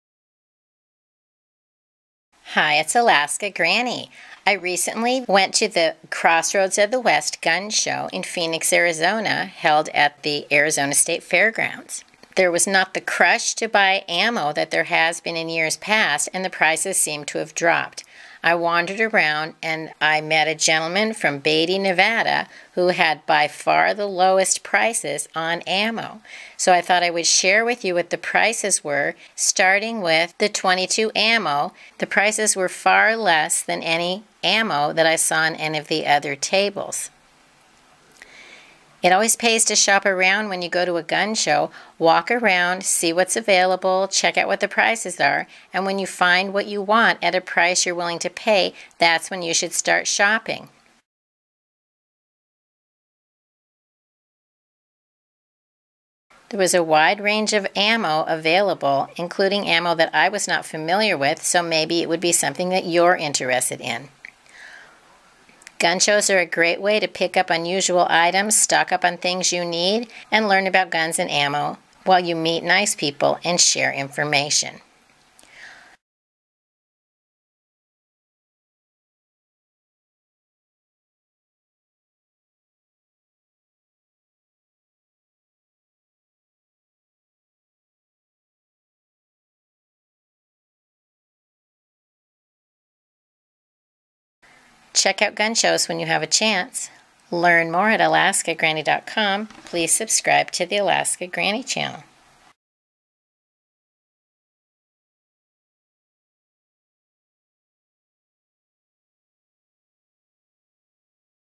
Arizona Ammo Prices Arizona Crossroads of the West Gun Show Review
Arizona Ammo Prices Arizona Crossroads of the West Gun Show Guide's Choice Holster Lots of ammo was available for sale at the Crossroads of the West Gun Show.  Selection varies, and prices seem lower than a year ago.    Check the availability and price of ammo in calibers you need when you shop.  Make a strategy to stockpile the ammo you want.  It won't go bad, and you have what you need when you want it. Prioritize the calibers you want to have on hand, and set a budget for the price you can afford.  As a responsible gun owner, it is your responsibility to keep guns and ammo away from those who should have no access.

★☆★ RECOMMENDED RESOURCES: ★☆★
Ammo Cans
Ammo Box
Shotshell Ammo Case